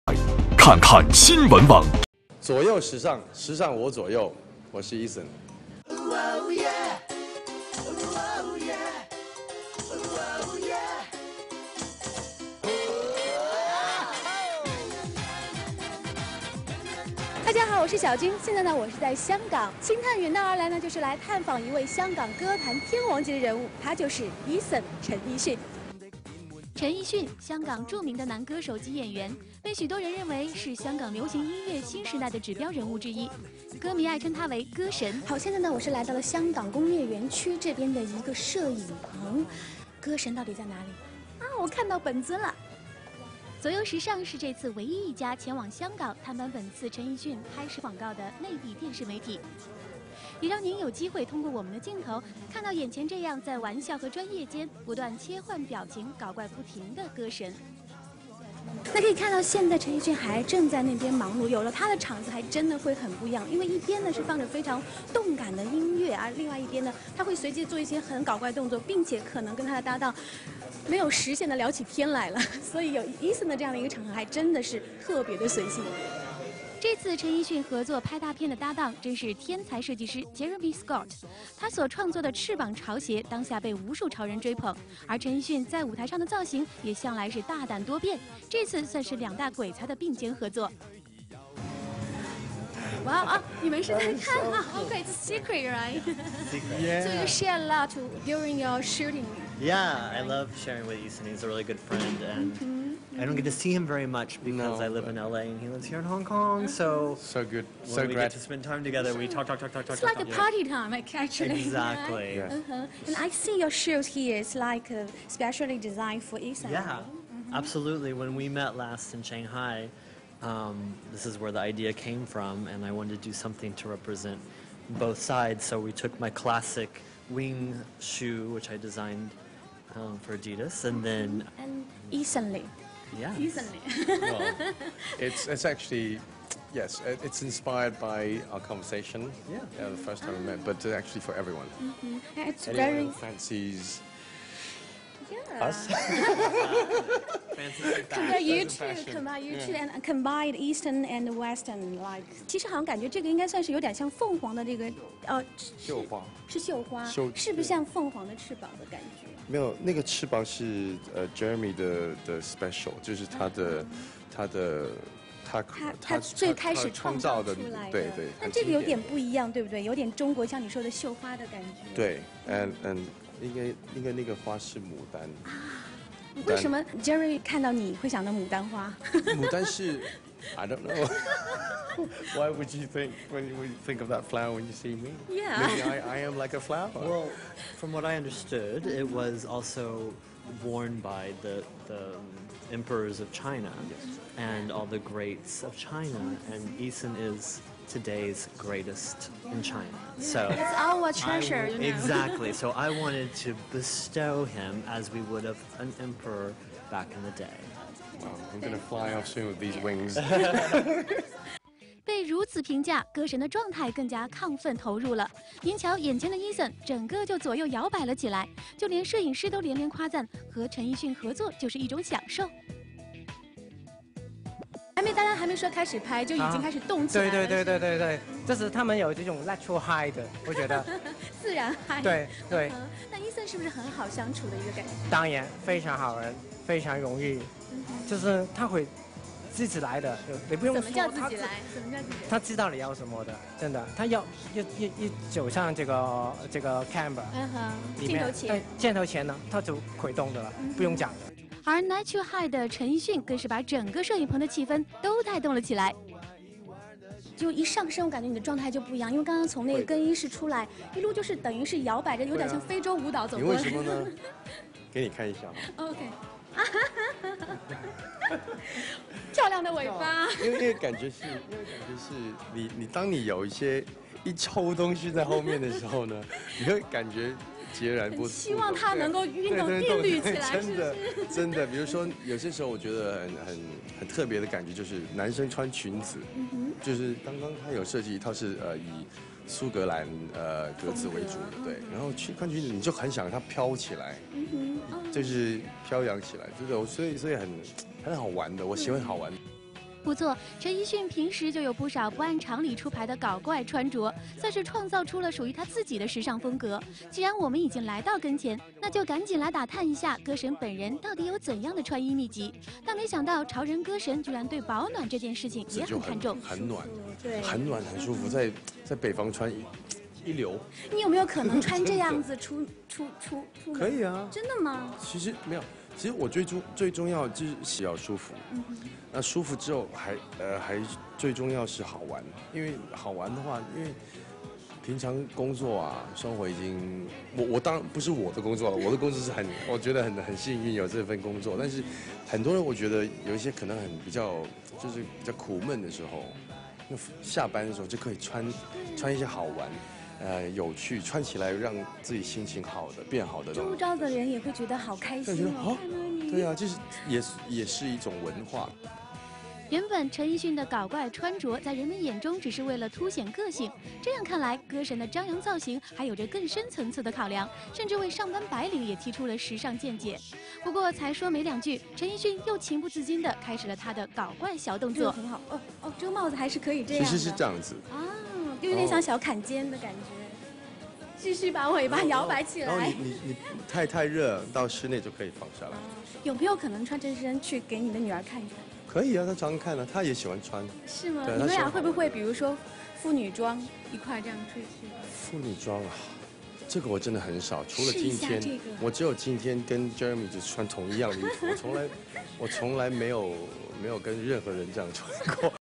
独家探班歌神 陈奕迅不改搞怪本色
陈奕迅，香港著名的男歌手及演员。被许多人认为是香港流行音乐新时代的指标人物之一，他亦被多数人认为是香港流行音乐泰山级人物张学友的接班人，香港乐坛新一代"歌神"。同时他也是继张学友后另一个在台湾获得成功的香港歌手，在2003年他成为了第二个拿到台湾金曲奖"最佳国语男演唱人"的香港歌手。陈奕迅曾被《时代》杂志形容为影响香港乐坛风格的人物。